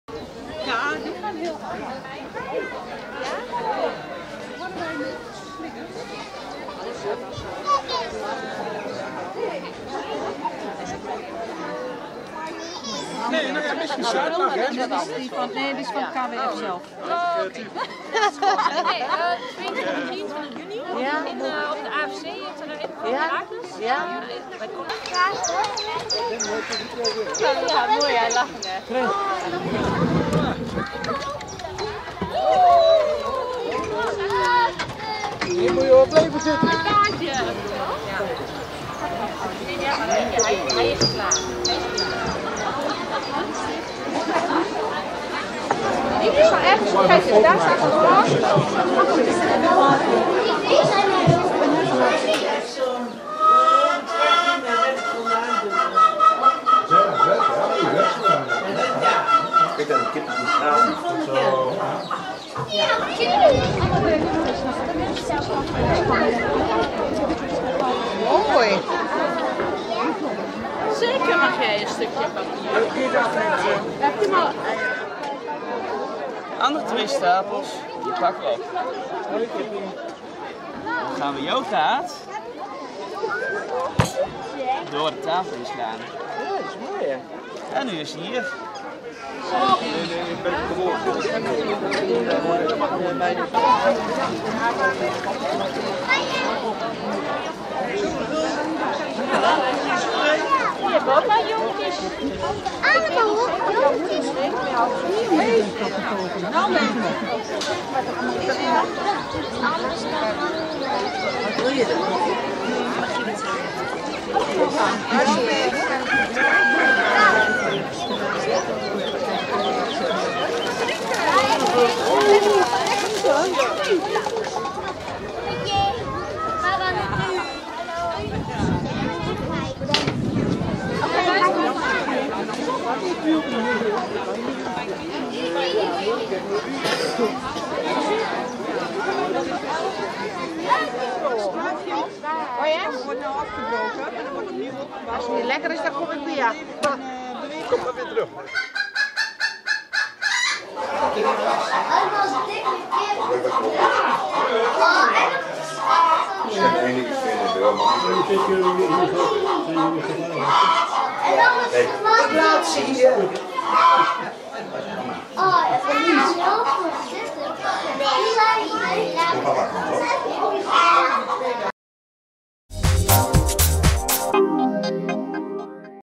Traditionele paasmarkt bij Stadsboerderij Beeklust
In de week voor Pasen is Stadsboerderij Beeklust het toneel voor de traditionele Paasmarkt. Behalve paasstukjes en voorjaarsplantjes waren er ook tal van activiteiten.

Zo was er een goochelshow, konden kinderen een ritje maken met een ponykar of op de foto met de paashaas. Uiteraard kregen de dieren op de stadsboerderij ook voldoende aandacht.